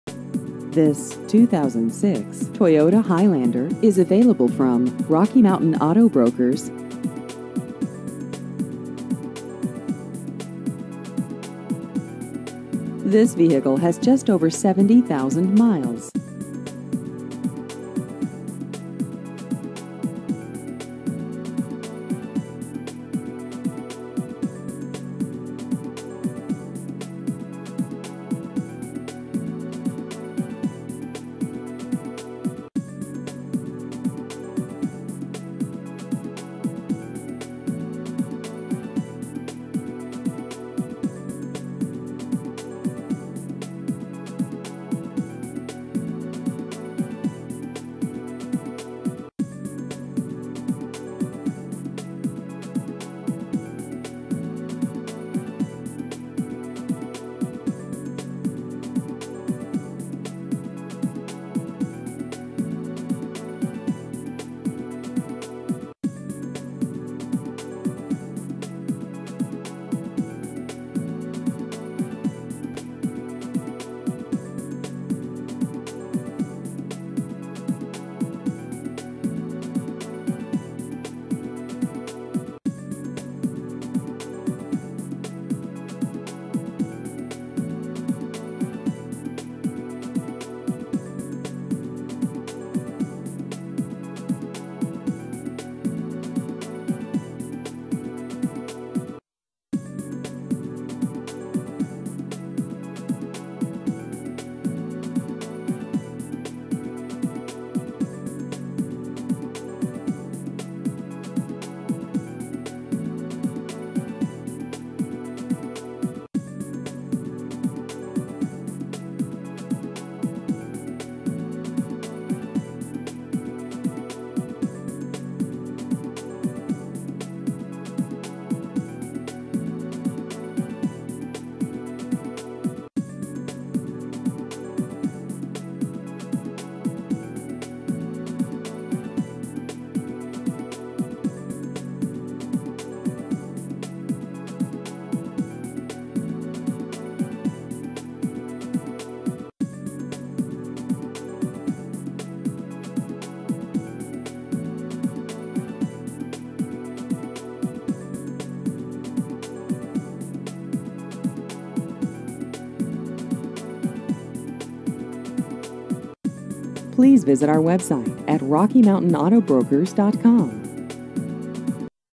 2006 Toyota Highlander Used Cars Colorado Springs CO
This 2006 Toyota Highlander is available from Rocky Mountain Auto Brokers.